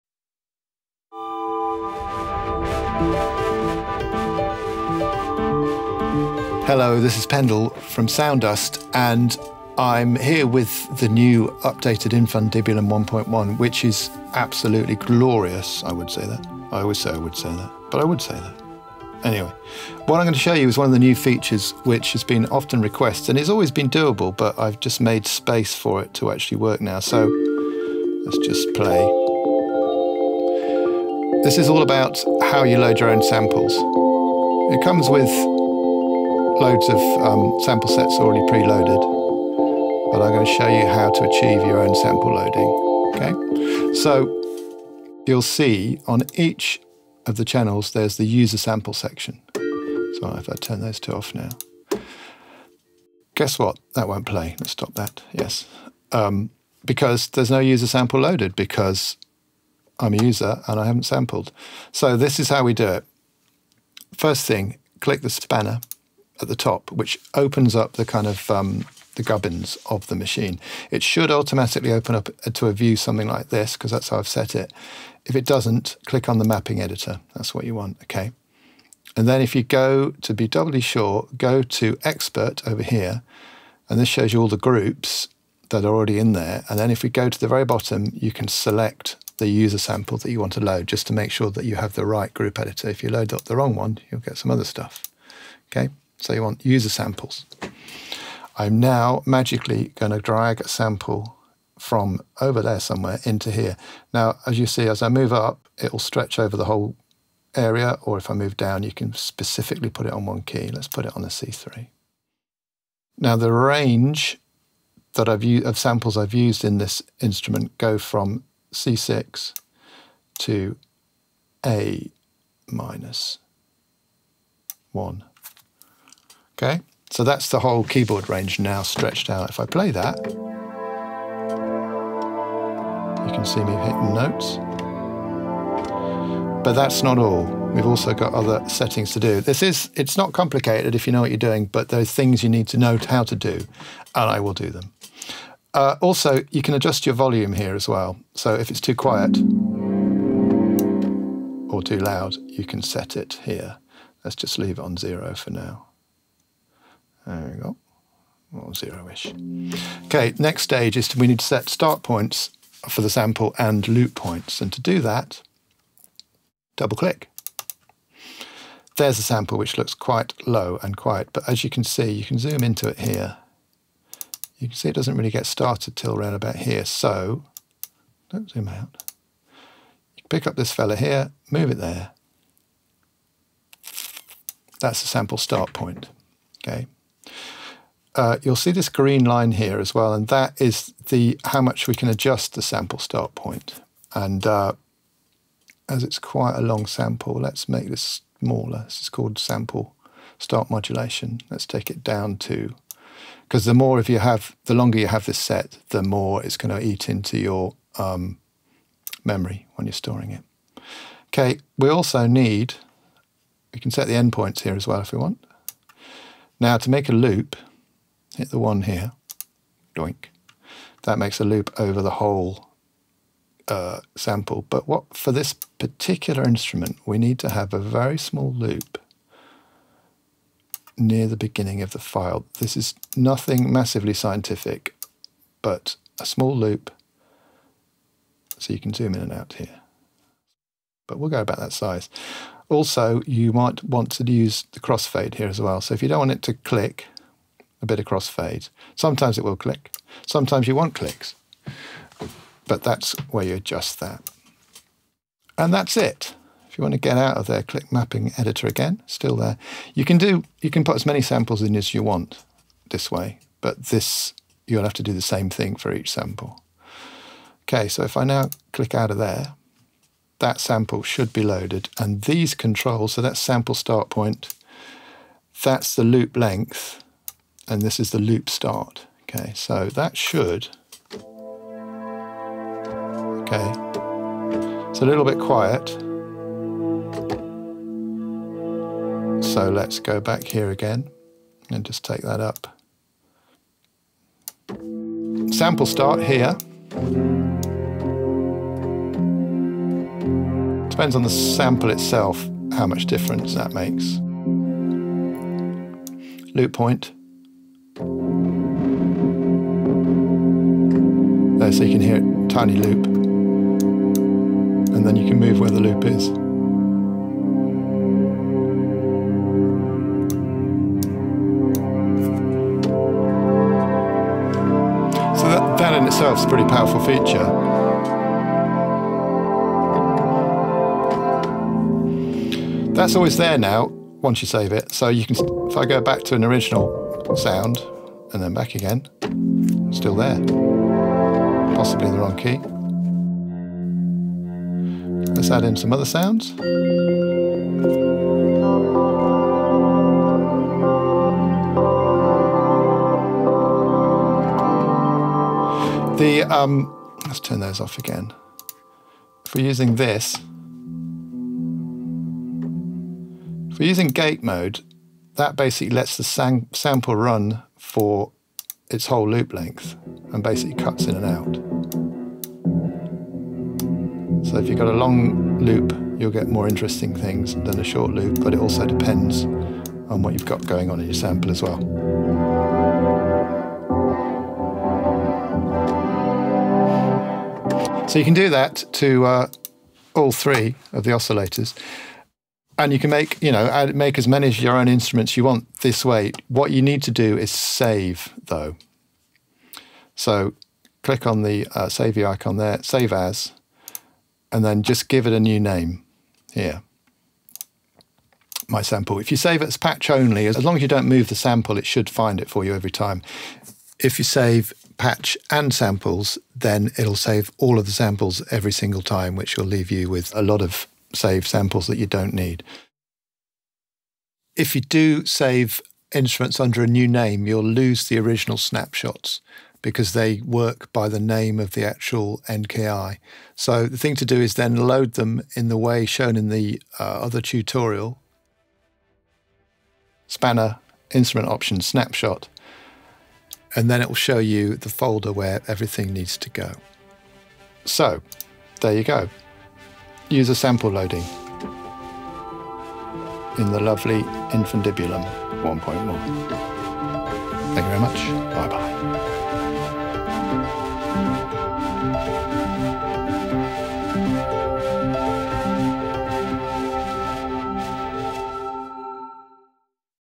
Sound Dust INFUNDIBULUM#1 1 - importing user samples into Kontakt
SOUND DUST INFUNDIBULUM#1.1 a new time funnel sample instrument for Kontakt.

This video shows you how to use the new sample loading function to add your own funny noises.